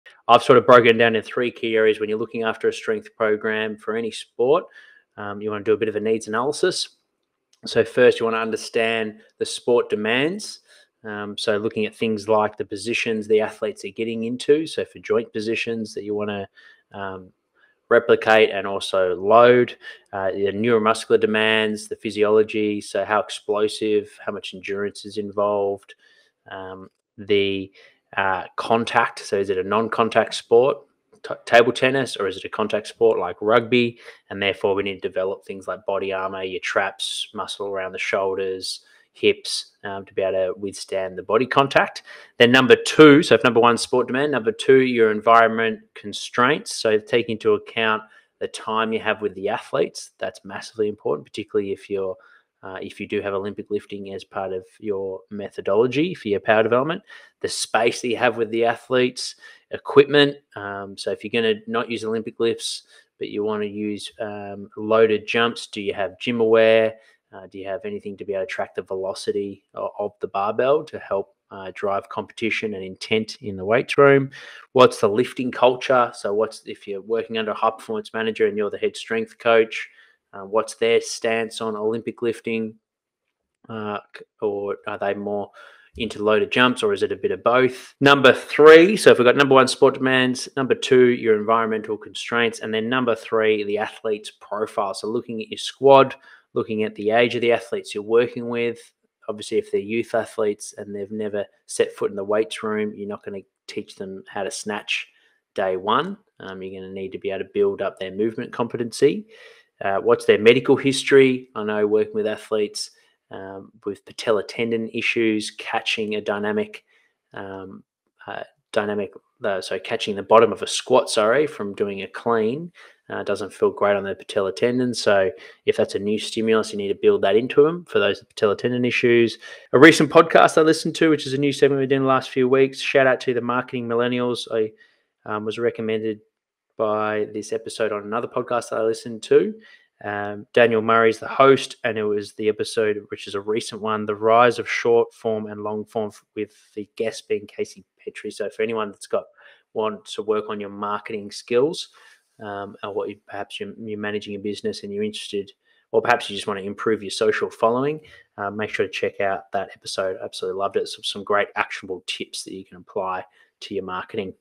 #73 - Olympic Lifting VS Loaded Jumps and Throws: What’s Better for Power Development
Highlights from the episode:

00:00 Sport Demands
00:42 Environment Constraint
01:28 Athletes Profile
02:10 Podcast recommendation ft. Marketing Millenials

The video discusses three key areas for developing power in a strength program for any sport: understanding sport demands, considering environmental constraints, and analyzing the athlete's profile. Understanding sport demands involves replicating joint positions and neuromuscular demands relevant to the sport, considering whether it's a contact or non-contact sport. Environmental constraints refer to factors like time, space, and available equipment, including the lifting culture and the preference for Olympic lifting or loaded jumps. Analyzing the athlete's profile includes assessing their age, movement competency, and medical history, as well as adapting training accordingly. The transcript briefly mentions a podcast episode related to marketing and business improvement, with actionable tips for marketing skills and social following.